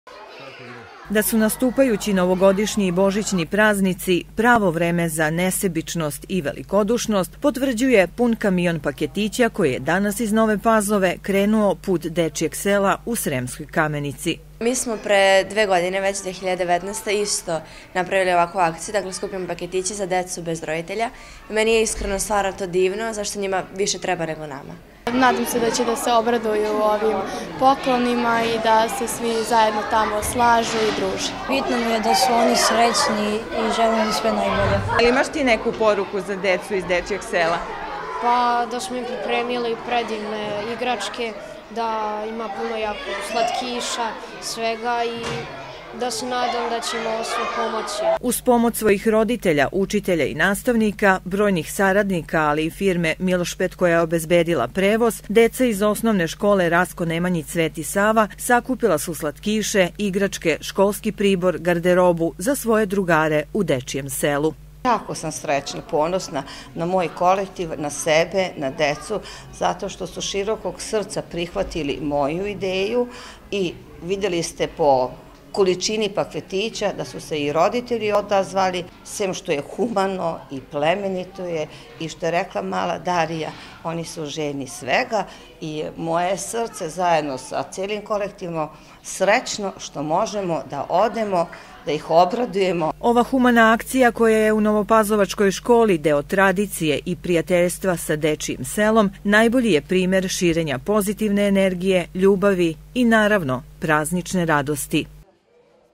Novopazovački osnovci novogodišnjim paketićima obradovali drugare iz Dečijeg sela u Sremskoj Kamenic
Predstavnici  Osnovne škole Rastko Nemanjić Sveti Sava iz Nove Pazove danas su uručili paketiće povodom novogodišnjih i  božićnih  praznika prijateljima u Dečijem  selu u Sremskoj Kamenici.